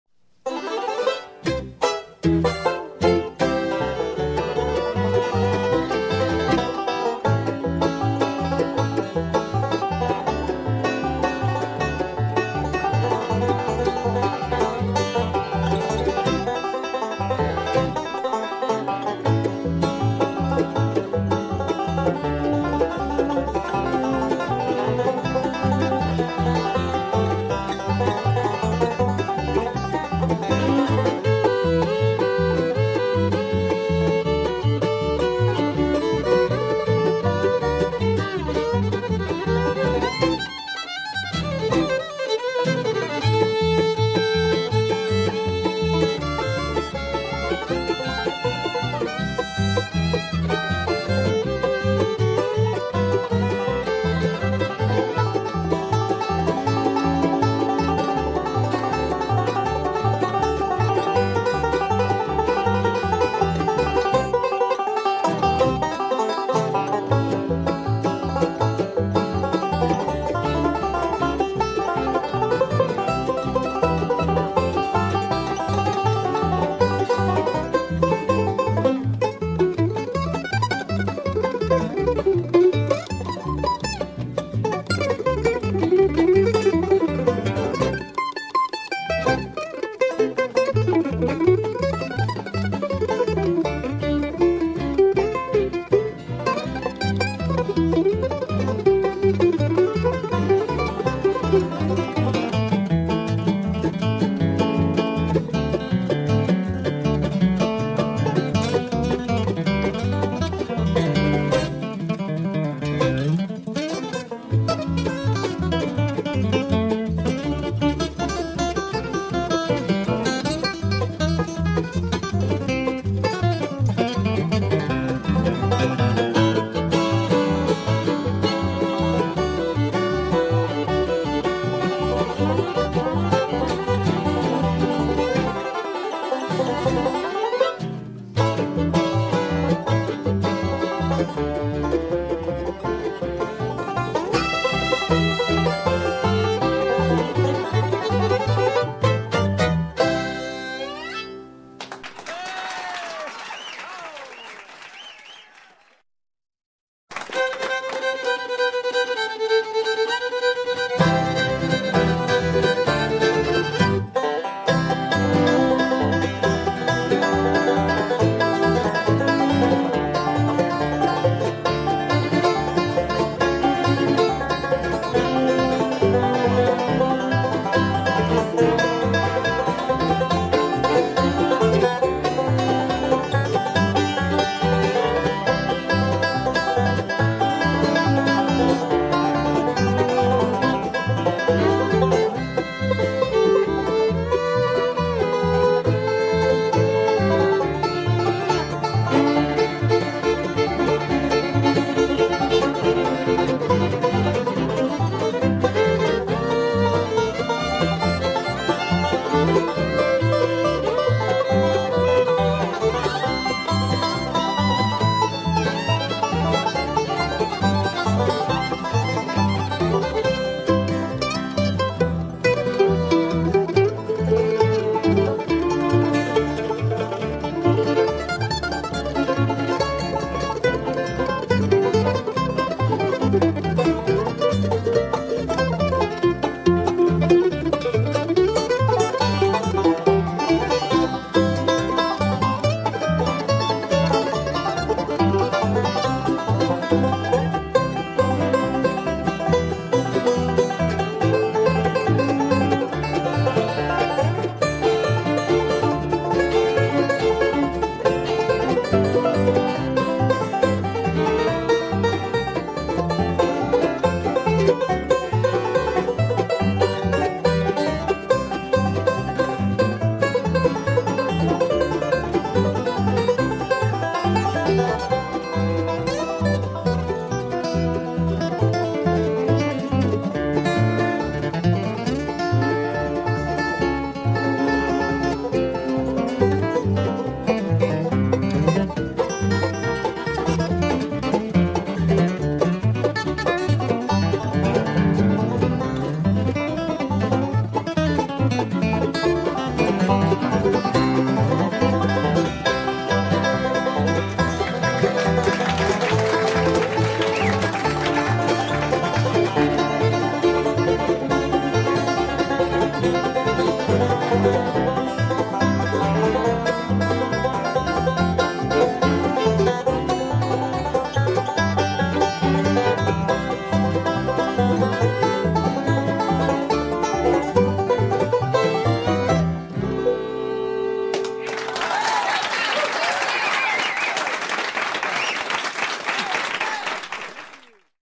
Pine Mountain - Slideshow
The Pine Mountain Bluegrass Jamboree is held each month in Remlap, Alabama and is "Free" to the public with limited seating. These photos are from the June 26, 2010 event.